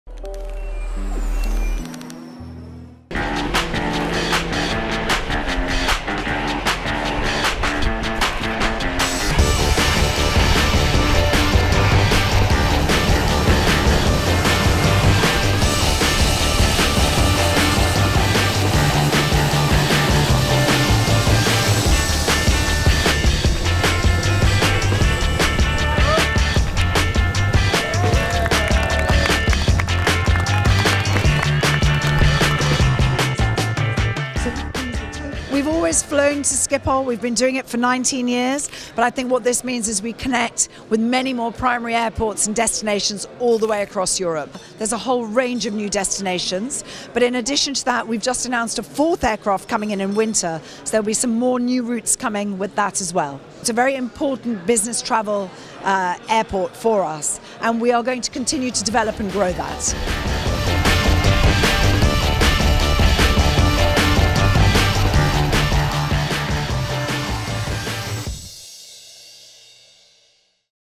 easyJet opens base at Schiphol
Jos Nijhuis and Maarten de Groof welcomed easyJet at Schiphol on Tuesday March 31. EasyJet has been flying to Schiphol for 19 years and now they have opened their own base here. Besides a new office and crew room, three planes have been stationed at Schiphol and another one will follow in Winter 2015.